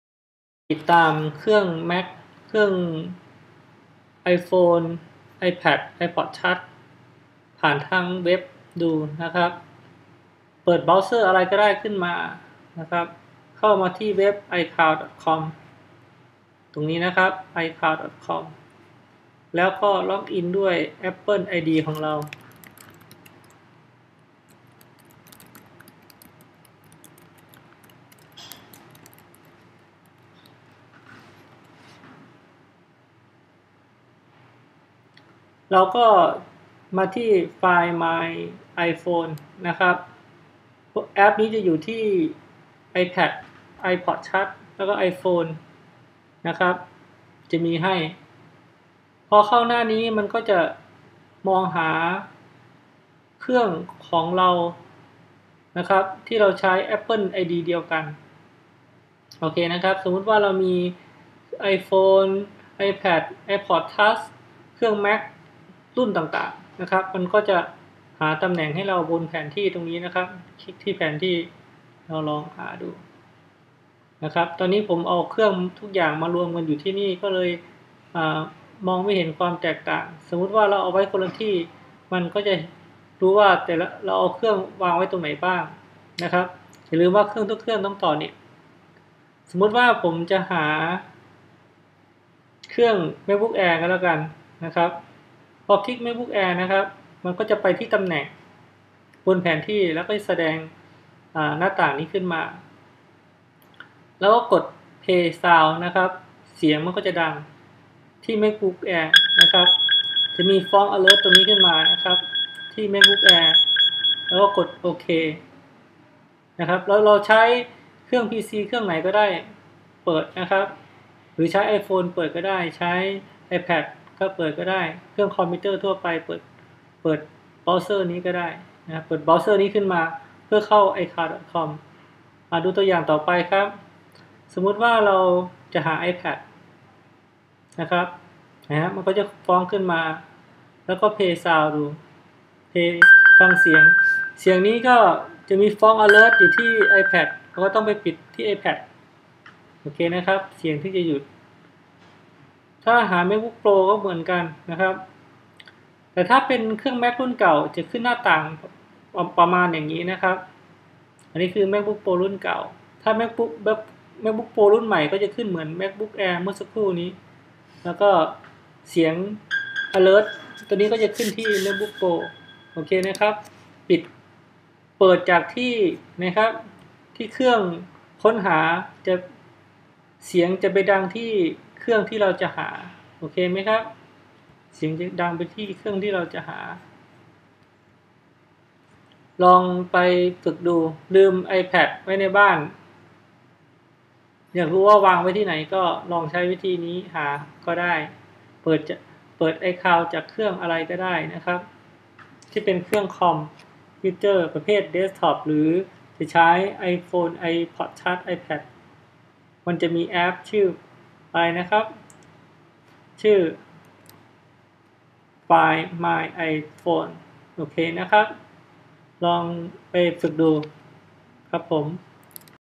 หาเครื่อง Mac, iPhone
เคยลืมหรือวางเครื่องไว้ที่ไหนเพียงต่อเน็ตก็หาได้เลย